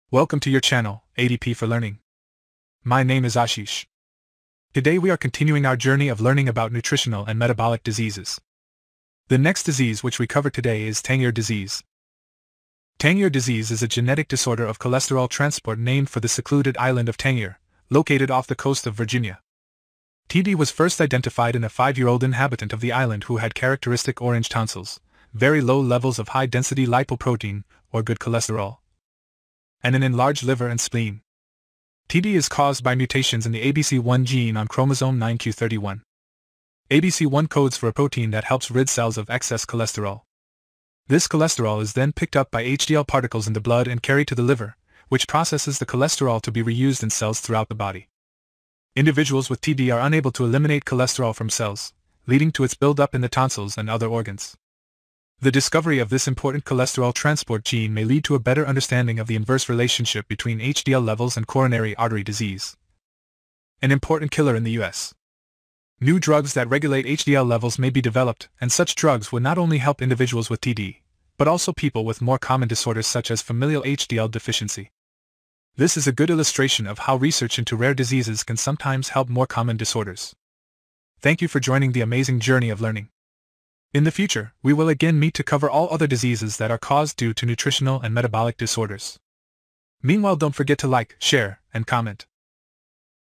Tangier Disease: Rare Genetic Disorder or Hidden Epidemic?
Tangier Disease is a rare genetic disorder that affects less than 1 in 1 million people worldwide. But could it be more common than we think? In this video, we explore the question of whether Tangier Disease is a rare genetic disorder or a hidden epidemic that has gone unnoticed for years.

We begin by introducing Tangier Disease and discussing its symptoms, causes, and diagnosis. We then dive into the genetics of the disease, explaining how it is inherited and what the current research says about its prevalence in different populations.

Next, we examine the evidence that suggests Tangier Disease may be more common than previously thought. We look at studies that have found high rates of Tangier Disease in certain communities and discuss the possible reasons for this.

Finally, we discuss the implications of Tangier Disease being a hidden epidemic. We explore the challenges that patients with the disease face in getting an accurate diagnosis and accessing appropriate treatment. We also highlight the importance of raising awareness about Tangier Disease and supporting research efforts to better understand and treat this rare genetic disorder.

Overall, this video provides a comprehensive overview of Tangier Disease and raises important questions about its prevalence and impact on public health. Whether you're a healthcare professional, a patient, or simply curious about rare genetic disorders, this video is a must-watch. Don't miss out on the latest insights and research on Tangier Disease!